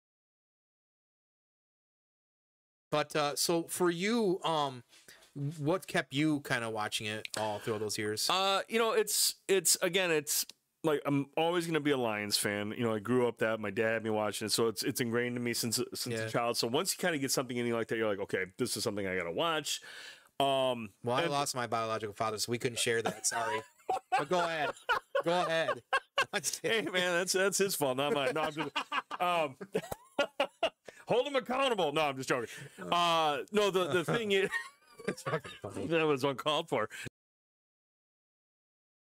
Watching the Lions with Dad?
Dude has never stopped watching the Detroit Lions or being a fan. Thanks to the many years of sharing the experience with his dad. Not all of us are that lucky, however.